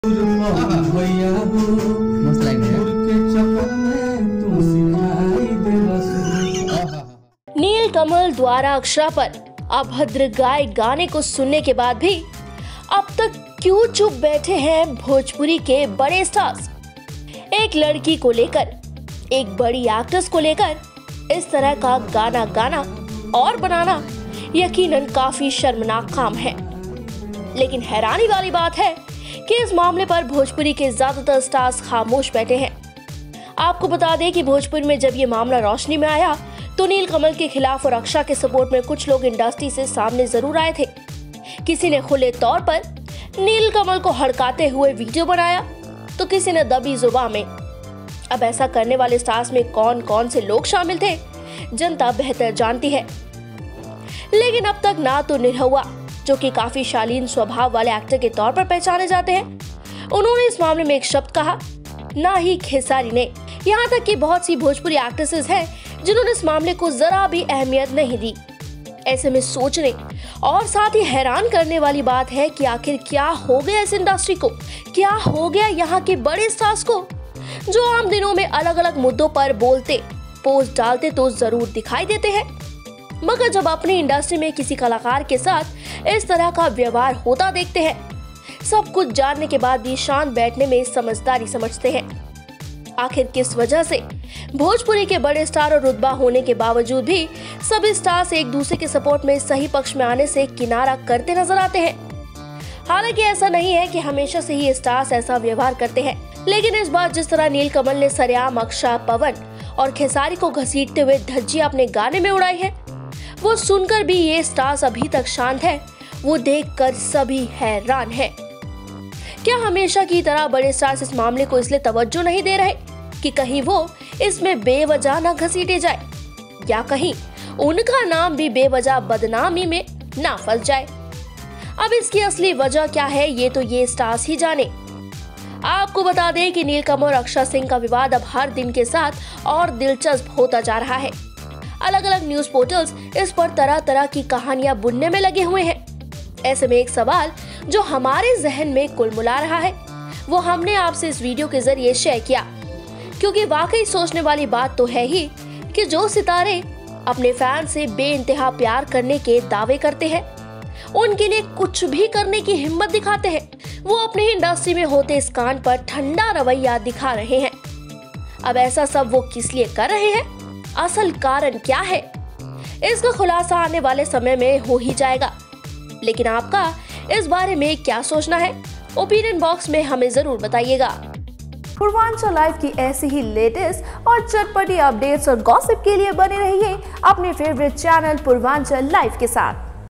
‘अक्षरा’ को बदनाम करने वालो के लिए चुपी साधे है, क्यों बैठे है भोजपुरी के बड़े स्टार्स !
Pawan Singh Khesari Lal Yadav # Akshara Singh Neel Kamal Singh
नीलकमल -अक्षरा विवाद पर अब तक क्यों खामोश है भोजपुरी के बड़े स्टार्स ?